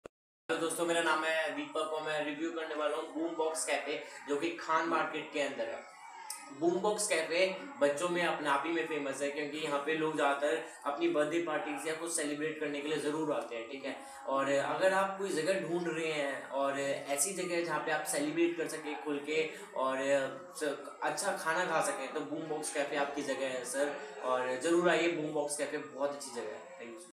Boombox Cafe , Delhi | Restaurants- American /Hookah Lounges /Pubs & Bars | askme.com
Boombox Cafe lies in Khan Market, New Delhi. They serve Chinese, Italian, North Indian, Lebanese and American delicacies. They have a chic ambiance along with mouth-watering Cocktails. Their Watermelon Hookah is extremely popular.